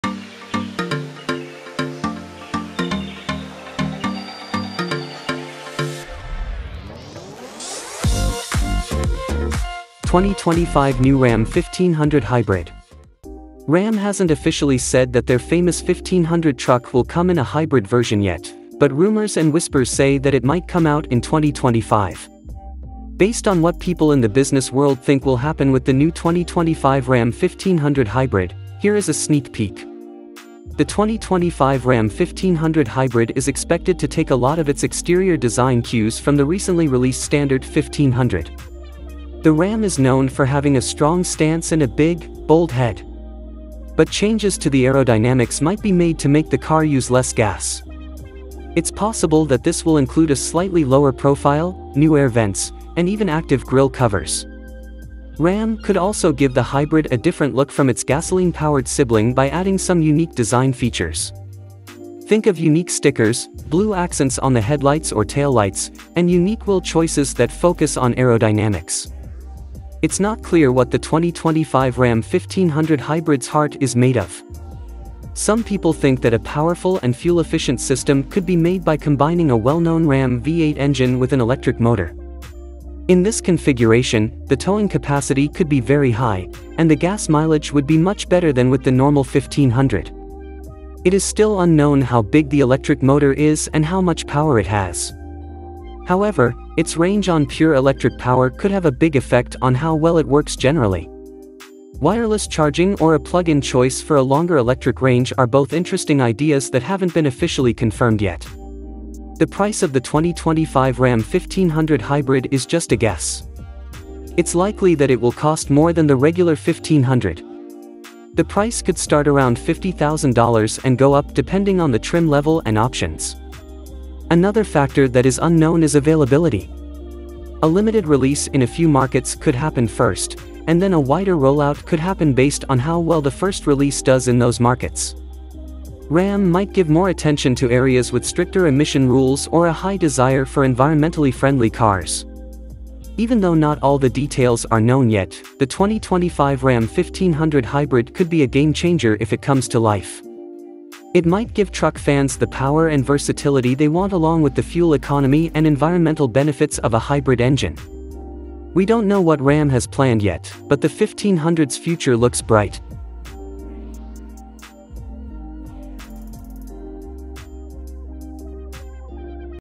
Game Changer! Unveiling the 2025 RAM 1500 Hybrid: Where Eco Meets Muscle!
Ram hasn't officially said that their famous 1500 truck will come in a hybrid version yet,  but rumors and whispers say that it might come out in 2025. Based on what people in the business world think will happen with the New 2025 RAM 1500 Hybrid,  here is a sneak peek.

The 2025 RAM 1500 Hybrid is expected to take a lot of its exterior design cues from the recently released standard 1500. The Ram is known for having a strong stance and a big,  bold head.  But changes to the aerodynamics might be made to make the car use less gas.  It's possible that this will include a slightly lower profile,  new air vents,  and even active grille covers.  Ram could also give the Hybrid a different look from its gasoline-powered sibling by adding some unique design features.  Think of unique stickers,  blue accents on the headlights or taillights,  and unique wheel choices that focus on aerodynamics.

It's not clear what the 2025 RAM 1500 Hybrid's heart is made of.  Some people think that a powerful and fuel-efficient system could be made by combining a well-known RAM V8 engine with an electric motor.  In this configuration,  the towing capacity could be very high,  and the gas mileage would be much better than with the normal 1500. It is still unknown how big the electric motor is and how much power it has.  However,  its range on pure electric power could have a big effect on how well it works generally.  Wireless charging or a plug-in choice for a longer electric range are both interesting ideas that haven't been officially confirmed yet.

The price of the 2025 RAM 1500 Hybrid is just a guess.  It's likely that it will cost more than the regular 1500. The price could start around $50,000 and go up depending on the trim level and options.  Another factor that is unknown is availability.  A limited release in a few markets could happen first,  and then a wider rollout could happen based on how well the first release does in those markets.  Ram might give more attention to areas with stricter emission rules or a high desire for environmentally friendly cars.

Even though not all the details are known yet,  the 2025 RAM 1500 Hybrid could be a game-changer if it comes to life.  It might give truck fans the power and versatility they want along with the fuel economy and environmental benefits of a hybrid engine.  We don't know what Ram has planned yet,  but the 1500's future looks bright.